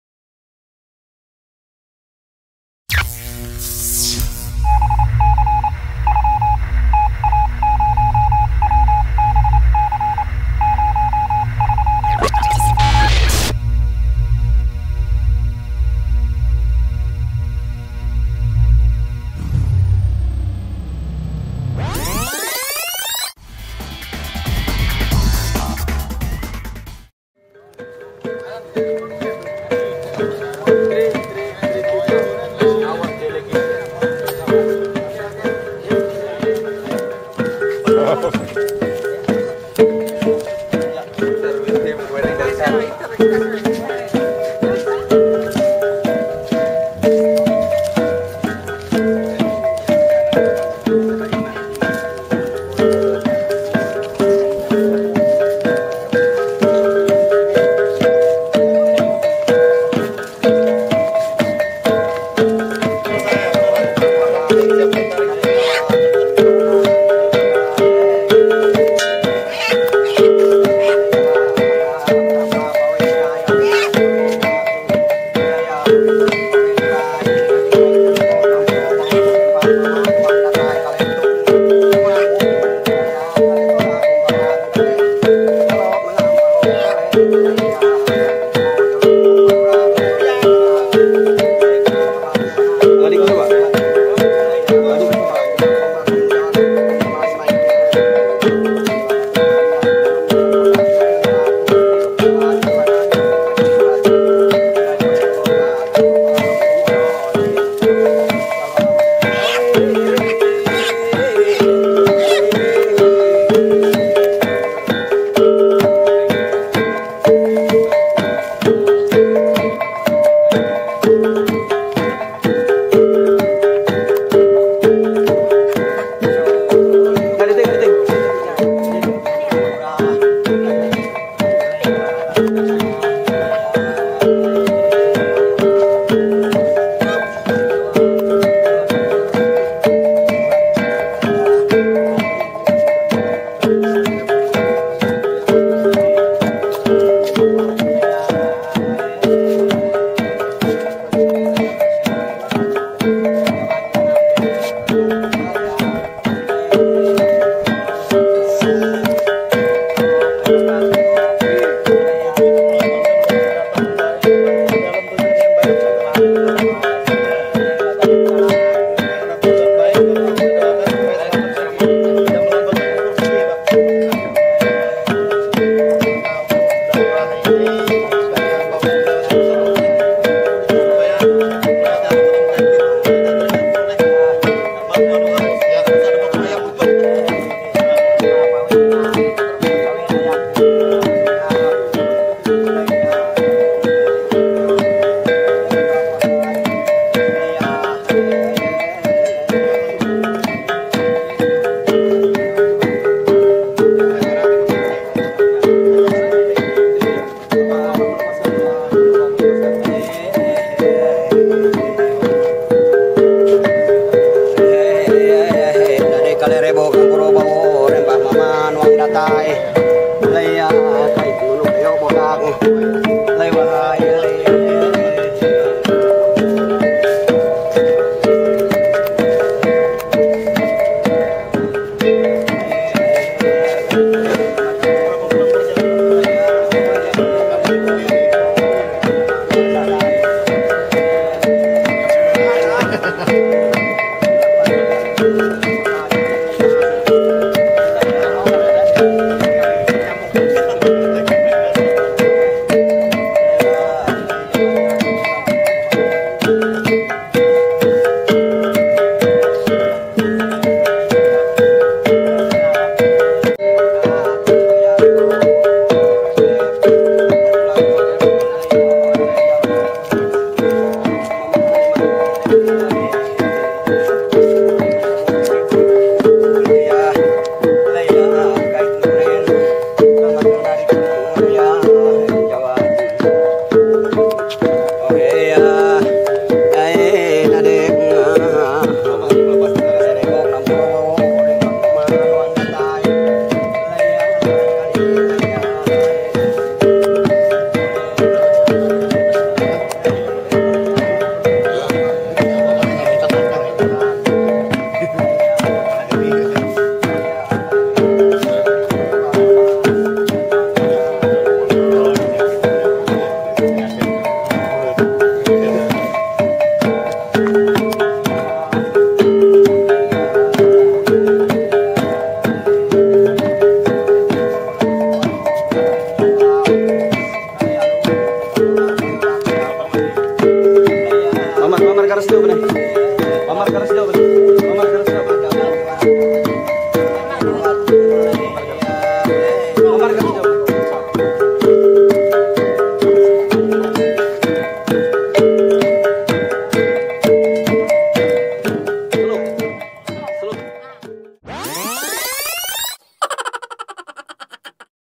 Borneo Expedition 09 Chicken Sacrifice
- Our Expedition across the Equator in Borneo 09.  A chicken was sacrificed to bless our trip.